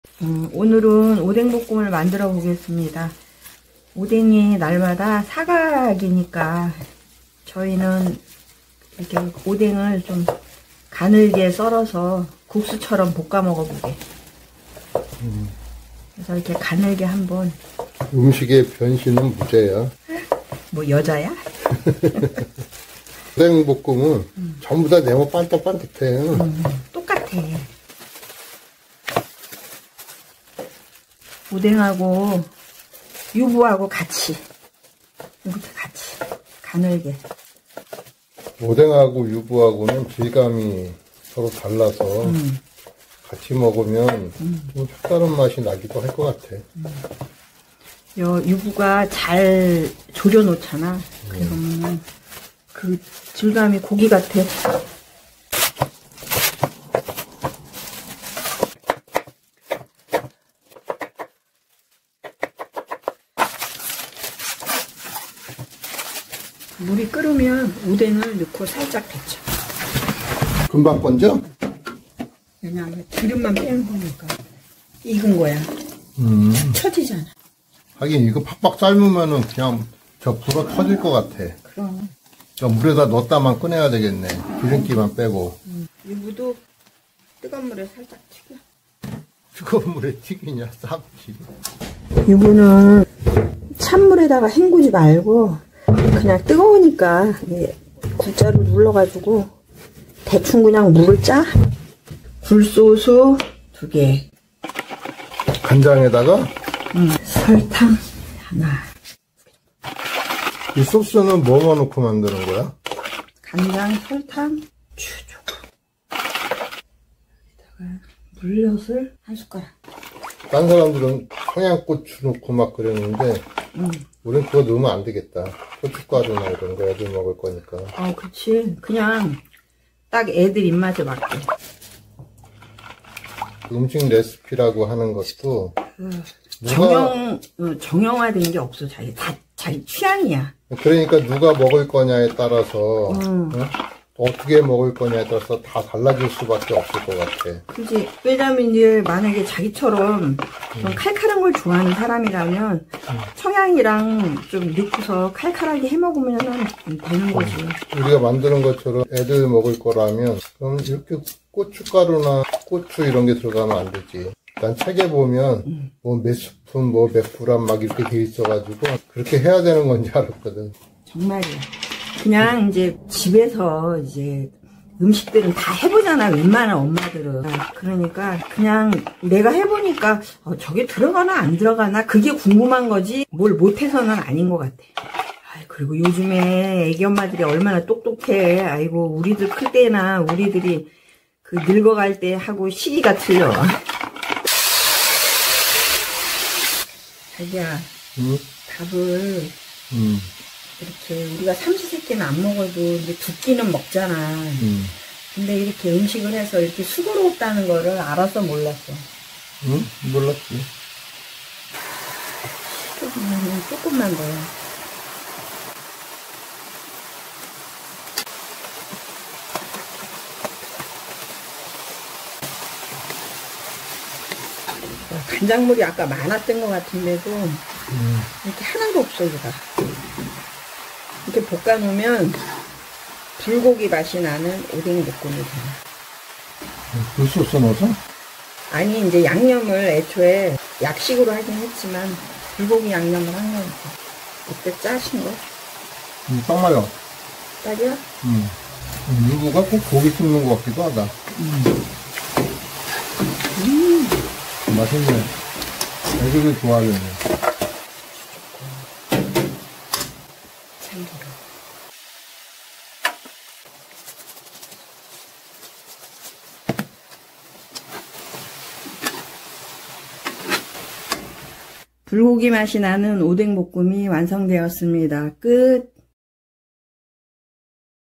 불고기 맛 나는 어묵볶음 맛있게 만드는법 유부 어묵 볶음
유부를 섞어 식감까지 올린 어묵볶음 만드는법 오뎅볶음